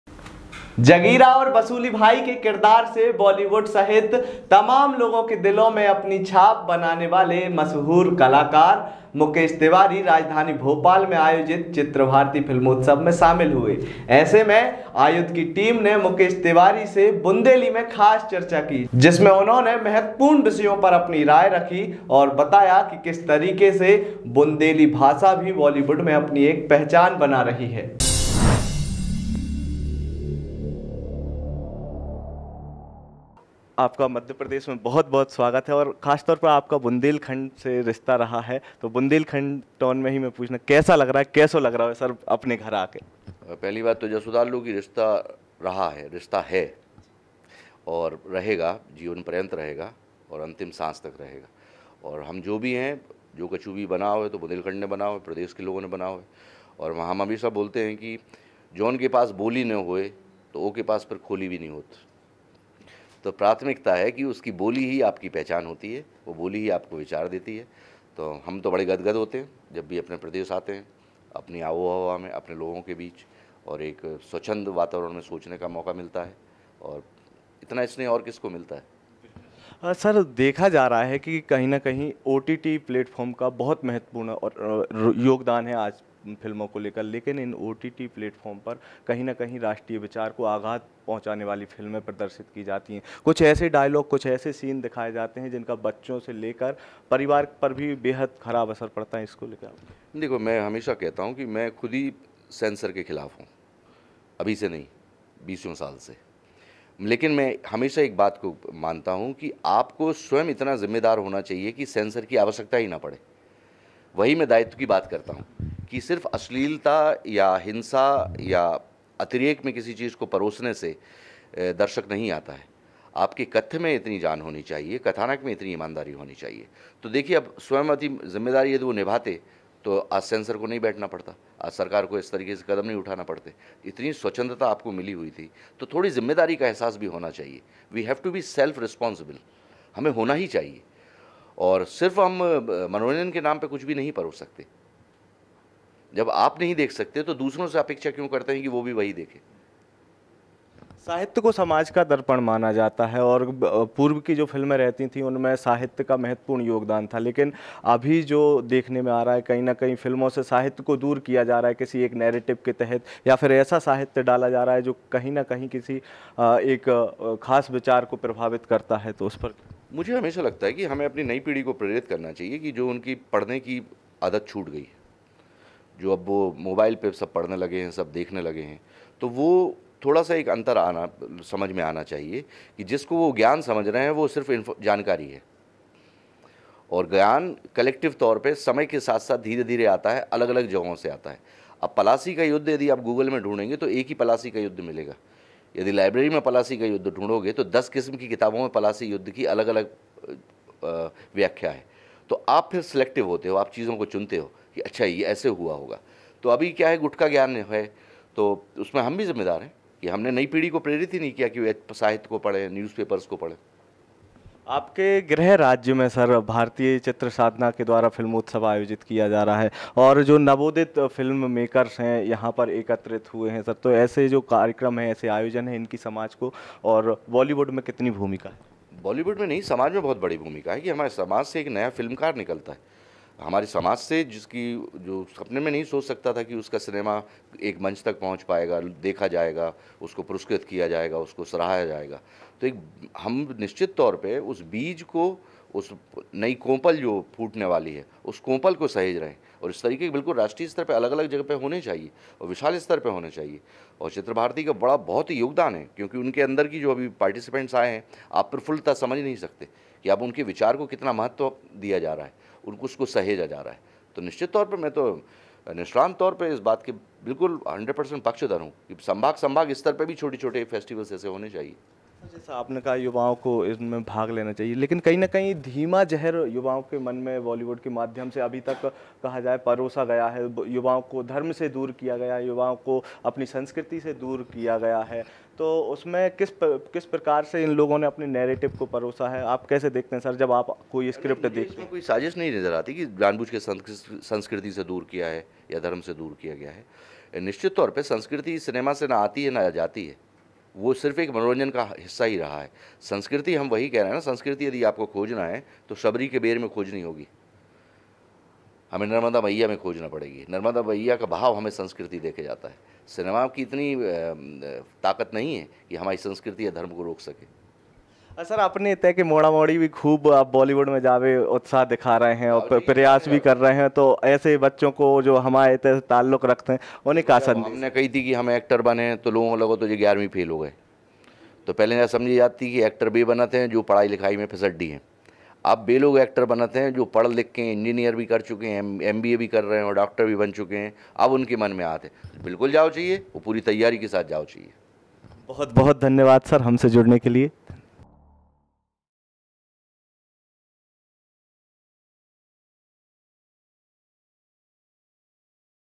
Mukesh Tiwari : भारतीय सिनेमा के Actor Mukesh Tiwari को बुंदेली में जबरदस्त Interview | AAYUDH
मध्यप्रदेश के सागर जिले से ताल्लुक रखने वाले अभिनेता मुकेश तिवारी भारतीय चित्र साधना द्वारा आयोजित फिल्म फेस्टिवल में शामिल होने भोपाल पहुचे इस दौरान उन्होंने aayudh से की बुंदेली में की जबरदस्त चर्चा
देखें पूरा वीडियो
सोशल मीडिया प्लेटफॉर्म्स पर हमसे जुडें और हमारा उत्साह बढायें